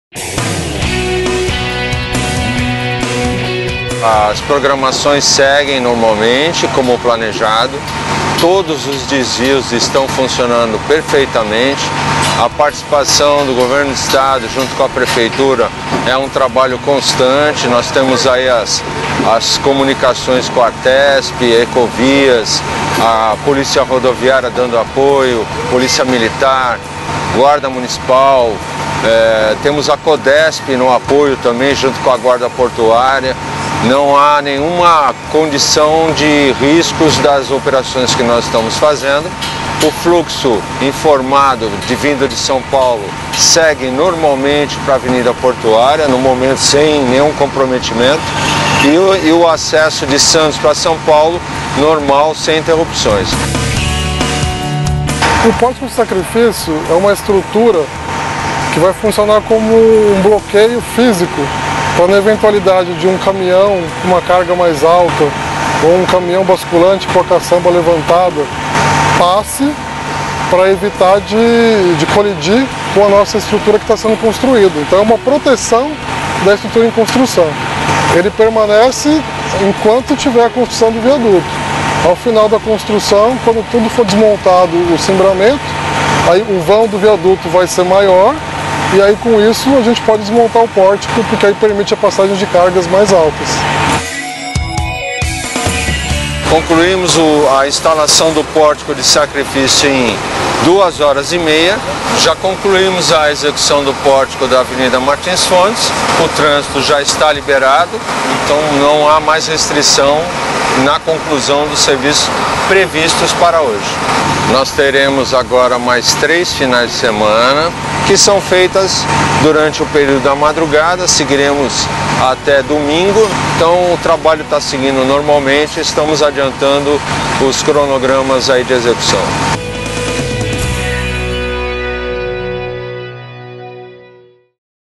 Pórticos são instalados para segurança da obra do viaduto na Nova Entrada de Santos
Duas estruturas metálicas limitadoras - com altura de 5,9m - foram implantados na entrada de Santos, neste final de semana. Eles vão garantir segurança nos trabalhos da próxima etapa da obra do viaduto que está sendo construído no encontro das avenidas Martins Fontes e Nossa Senhora de Fátima. Os pórticos serão provisórios e permanecerão até o término da obra do viaduto. Confira no nosso portal as interdições dos próximos fins de semana.